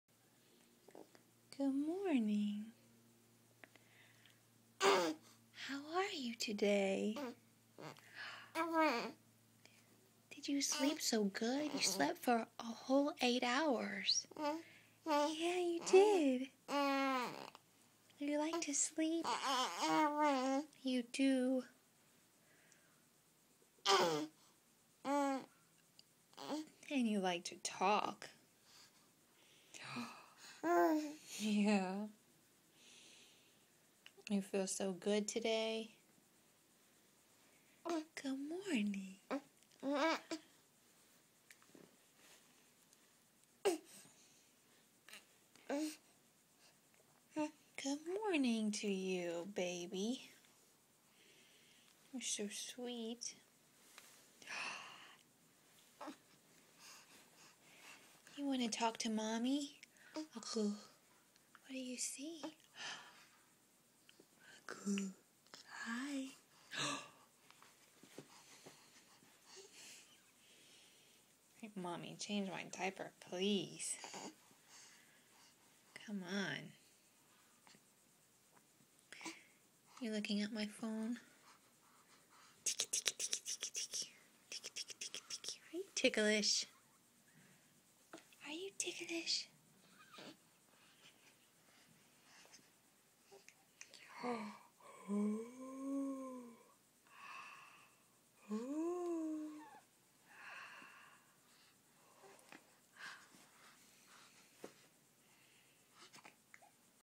Good morning...
After 8 hours of continuous sleep ( then 1:30 more) Elena feels great!